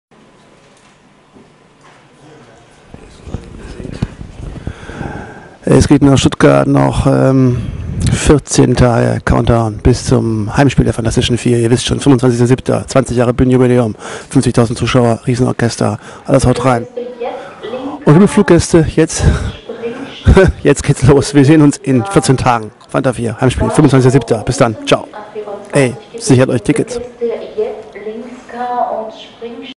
20 Jahre die Fantastischen Vier - nur noch 14 Tage
Nur noch 14 Tage, dann ist es soweit: Die Fantastischen Vier feiern ihr 20-Jähriges Bandjubiläum mit einem Orchester Live und Unplugged im Rahmen eines spektakulären Konzertes auf dem Cannstatter Wasen in Stuttgart.